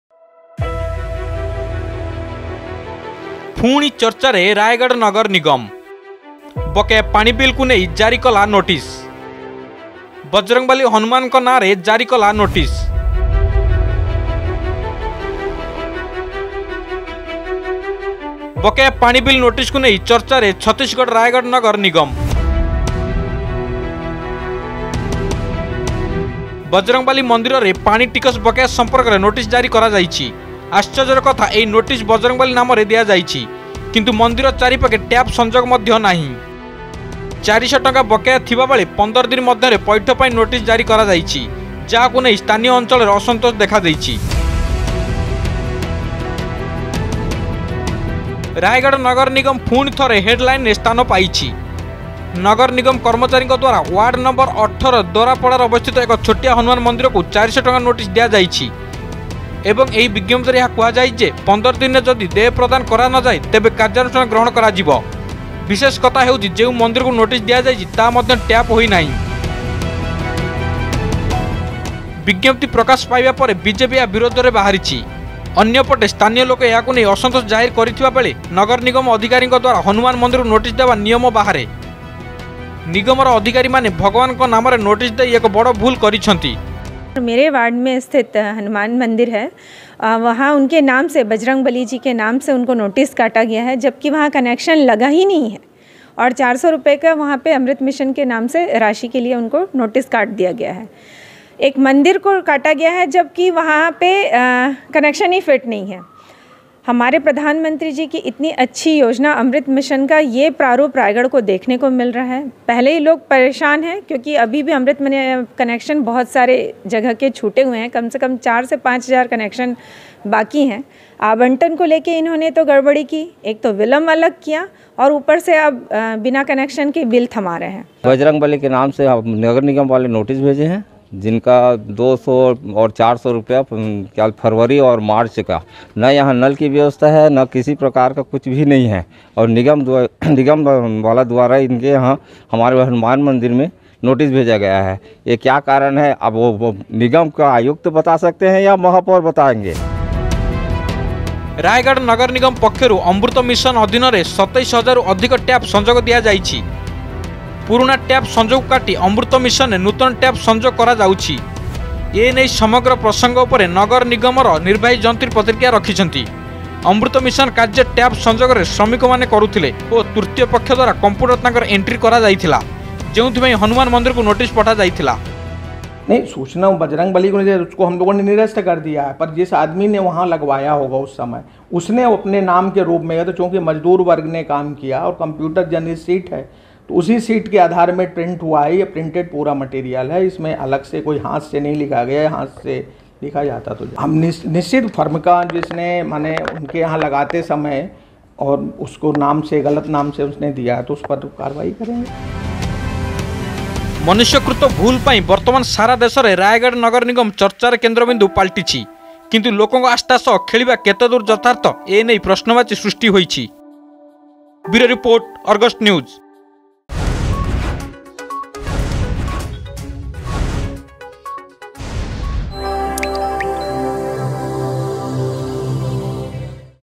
ପୁଣି ଚର୍ଚ୍ଚାରେ ରାୟଗଡ ନଗର ନିଗମ
ବକେୟା ପାଣି ବିଲକୁ ନେଇ ଯାରି କଲା ନୋଟିସ
ବଜରଙ୍ଗବାଲି ହନୁମାନଙ୍କ ନାଁରେ ଯାରି କଲା ନୋଟିସ
ବକେୟା ପାଣି ବିଲ ନୋଟିସକୁ ନେଇ ଚର୍ଚ୍ଚାରେ ଛତିଶଗଡ଼ ରାୟଗଡ ନଗର ନିଗମ । ପାଣି ବିଲ୍ ଜମା କରିବାକୁ ରାୟଗଡ ନଗର ନିଗମ ପକ୍ଷରୁ ବଜରଙ୍ଗବାଲିକୁ ନୋଟିସ୍ ପଠାଇଛି, ଏହା ଶୁଣିବାକୁ ଅଜବ ଲାଗୁଛି, କିନ୍ତୁ ରାୟଗଡ ନଗର ନିଗମ ଏହା କରିଛି ।